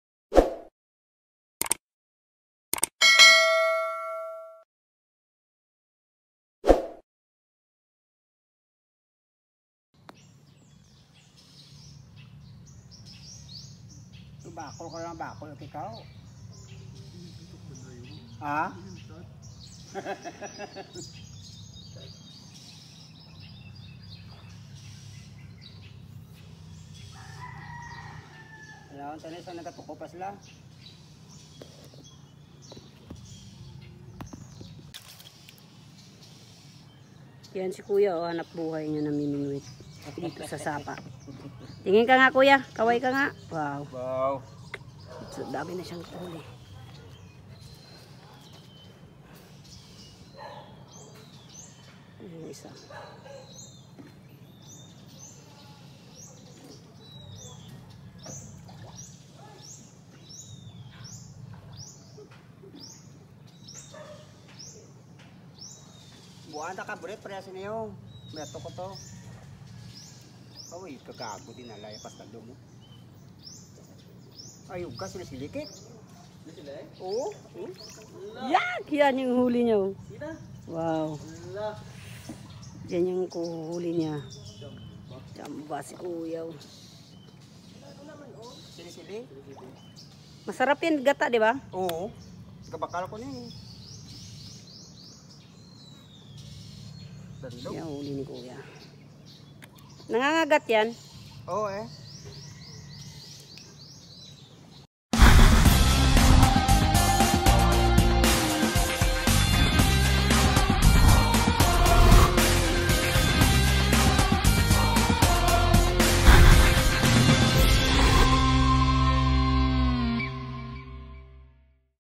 Tatay ng talisay namingwit ng silik
Nang huli ng tilapia sa sapa at silik..